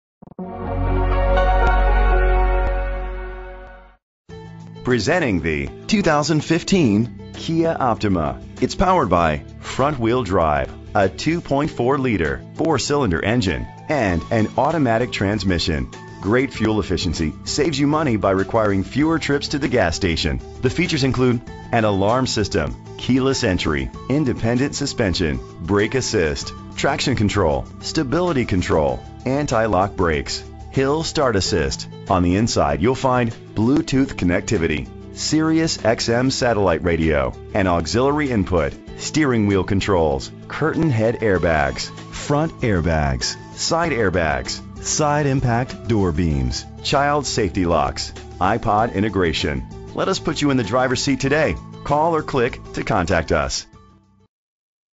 2015 Kia Optima KF6305 - Orland Park IL
Year: 2015
Make: Kia
Model: Optima
Trim: LX
Engine: 2.4 liter 4 cylinder FWD
Transmission: Automatic With Overdrive
Color: Silver
Mileage: 0
Address:
8100 West 159th StreetOrland Park, IL 60462
VIN:5XXGM4A75FG420639